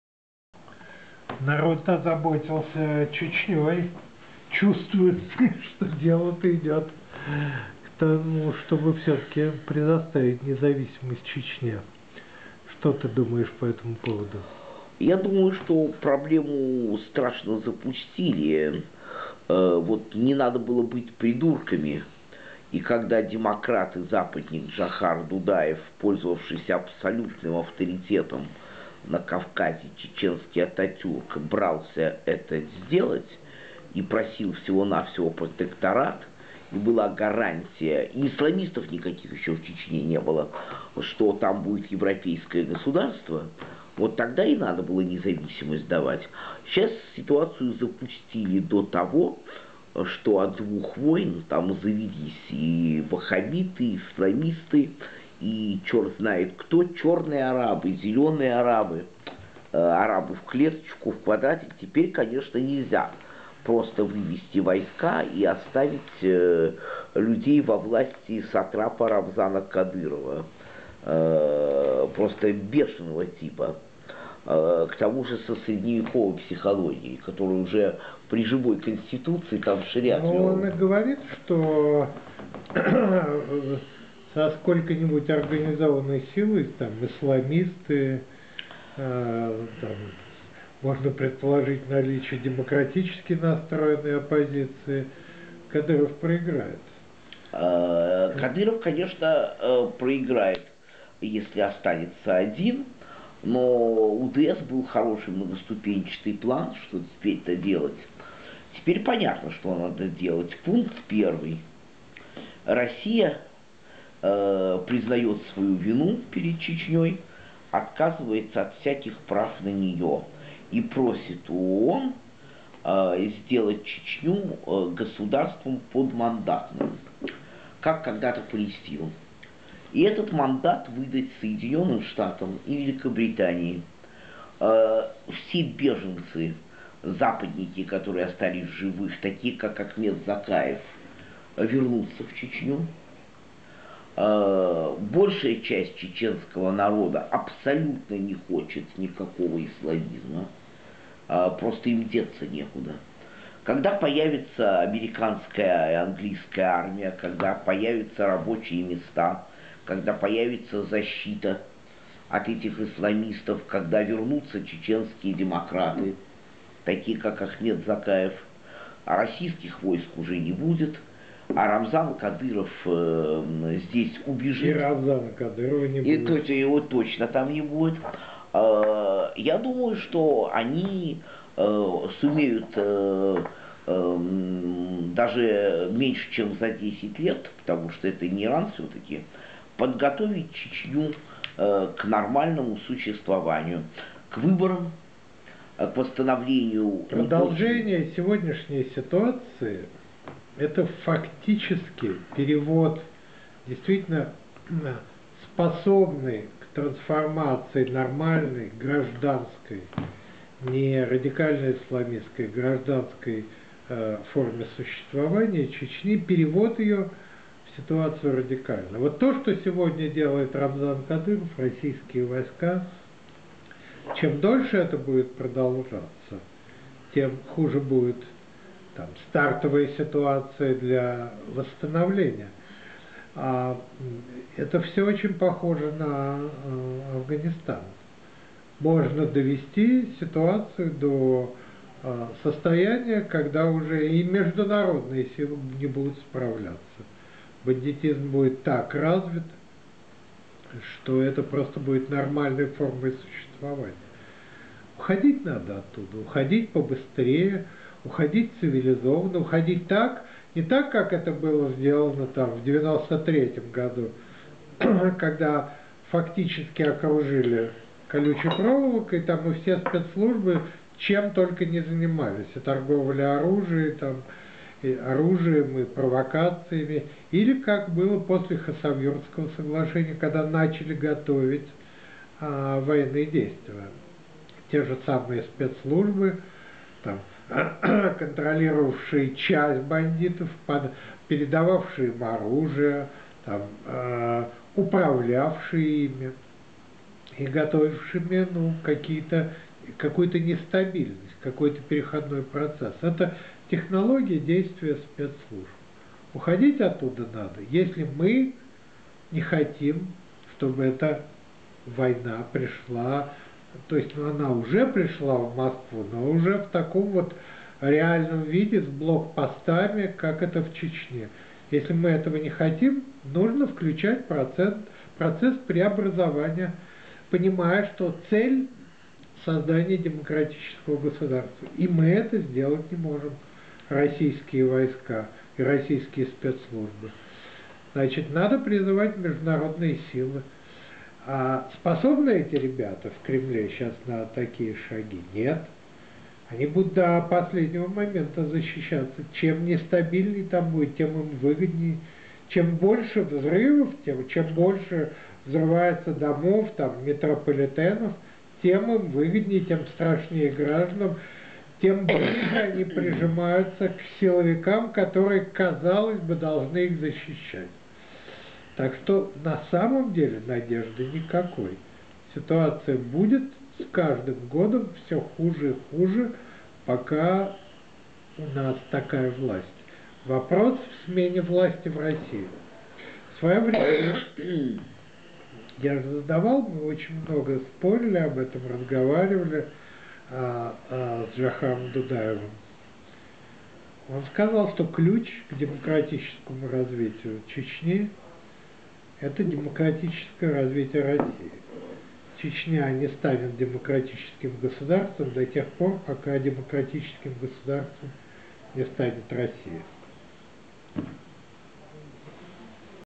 К чеченскому вопросу Боровой, Новодворская
К чеченскому вопросу
Пожалуйста, выскажите свою точку зрения на Чечню. Что делать? Можно ли уйти, предоставив чеченцам самим решать свою судьбу? Конечно, весьма заманчиво было бы перестать нести потери и избавить чеченцев от неудобств, налагаемых оккупацией. Но это наверняка вызовет в Чечне гражданскую войну между Кадыровым и исламистами, и Кадыров, скорее всего, проиграет. Во времена "первой чеченской республики" более-менее приемлемые политики - Масхадов, например - играли весьма скромную роль и не могли справиться с бандитами наподобие Басаева и Радуева (или не хотели, не знаю). Если вывести войска из Чечни, то скоро она станет ульем исламистов, которые не остановятся и попытаются также добиться ухода России из других республик Сев. Кавказа, а потом, возможно, с Кавказа вообще. Не исключено, что Чечня будет территорией беззакония, наводненной эмиссарами террористических организаций Ближнего Востока. Половина населения убежит в Россию. Неизвестно, можно ли выводом войск добиться прекращения терроризма. Вот евреи вывели свои войска из Газы, и стало только хуже. Как и в случае с Газой, чеченская проблема представляется мне нерешаемой. Прошу вас (с Боровым) высказать свое мнение.